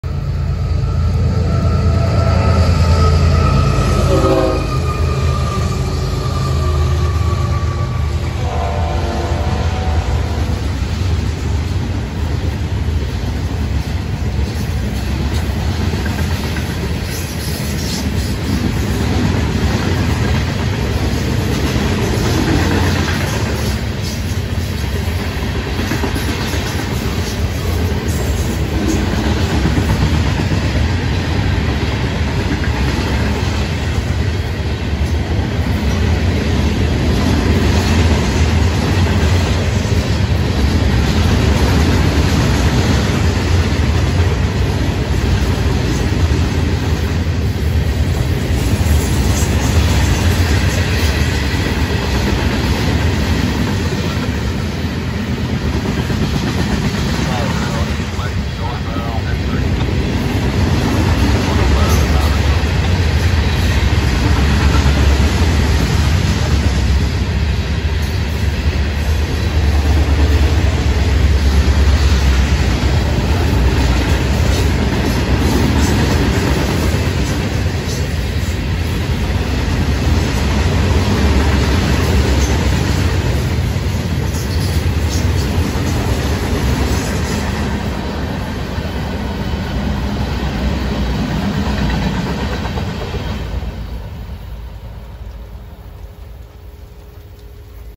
FEC TRAIN 206 @ 4:54pm 9-26-23
Northbound Fec engines 812 lng tender 306,&815 leading rock& fly ash